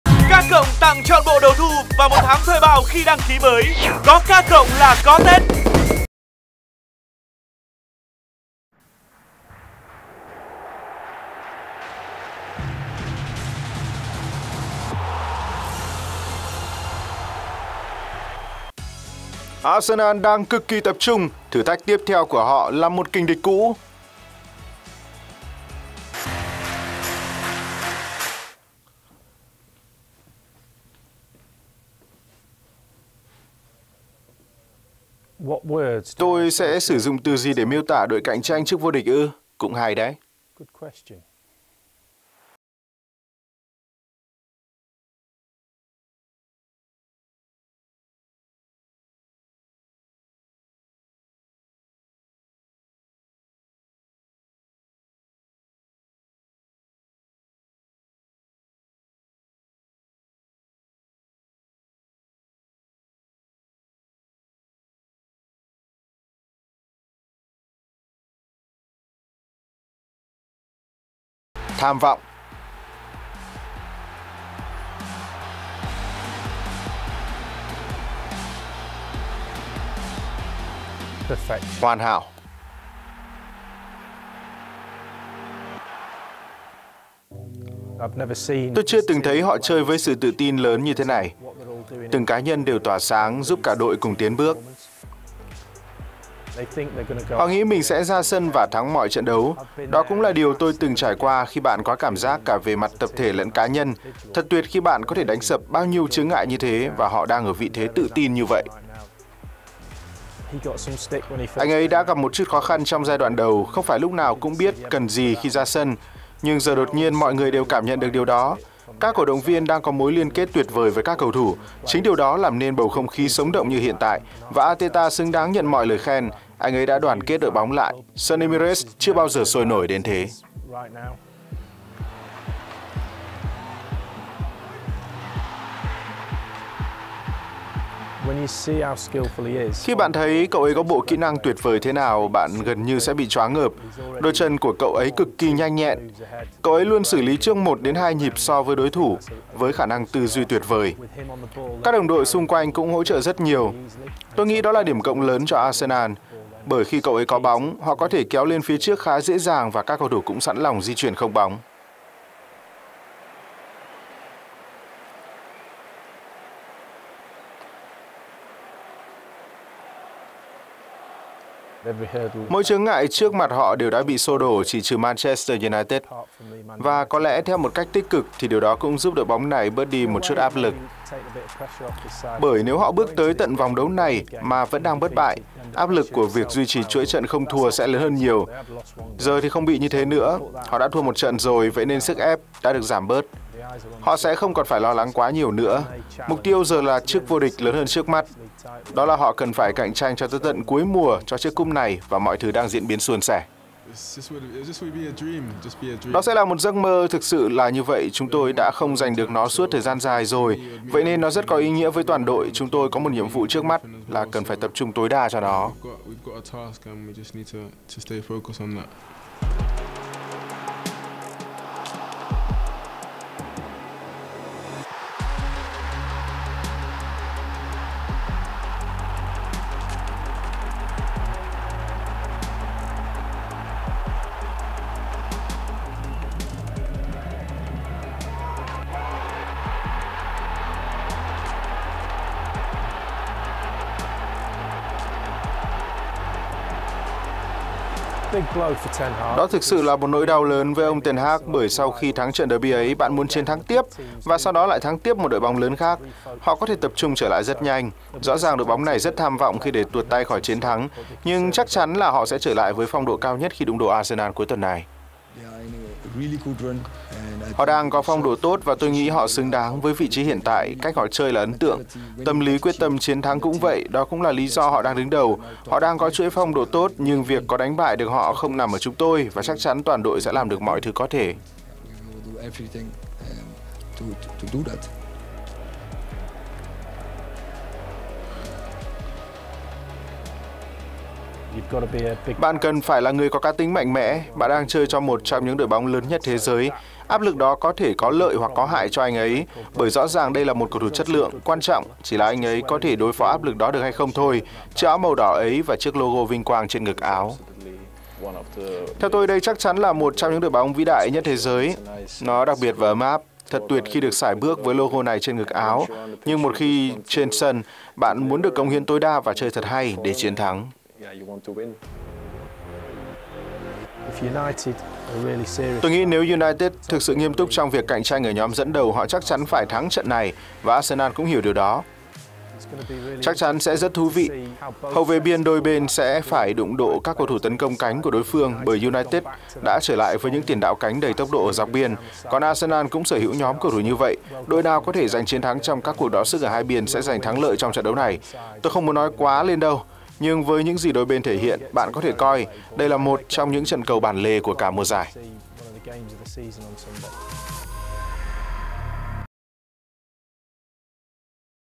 SUPER SUNDAY: ARSENAL - MAN UNITED | SẮC ĐỎ TRANH ĐẤU, TEN HAG ĐẤU ARTETA | NGOẠI HẠNG ANH 22/23
CÓ K+ LÀ CÓ TẾT !! ƯU ĐÃI ĐÓN TẾT 2023
Tặng trọn bộ đầu thu DTH (trị giá 1.200K) khi đăng ký mới Gói Trọn Vẹn (Từ 1/1 - 21/1)